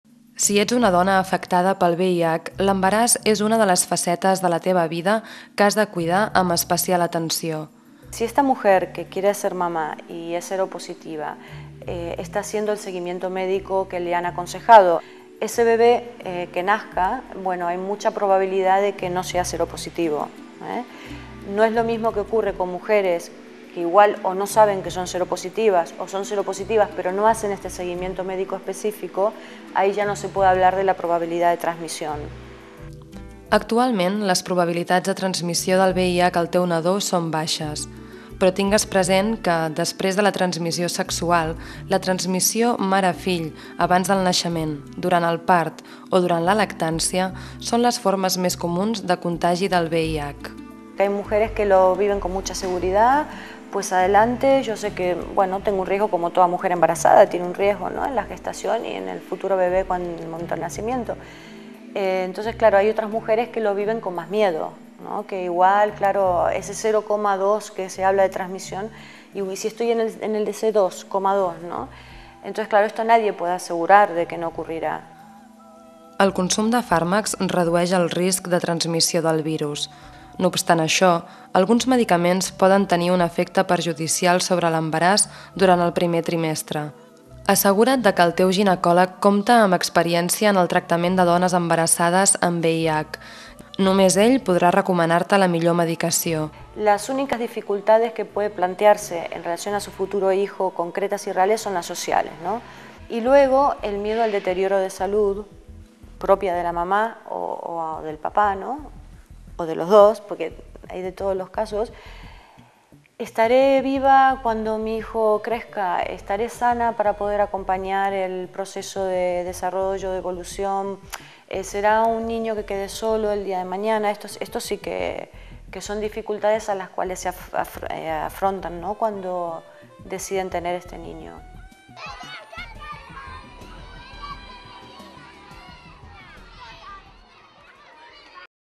VIU I CONVIU AMB EL VIH (Fragmento: El VIH i la maternitat)
4L Comunicación realiza los contenidos y el off de la Guía Audiovisual Interactiva Viu i Conviu amb el VIH, proyecto de la productora Blauhaus Audiovisual y subvencionado por el Ayuntamiento de Barcelona.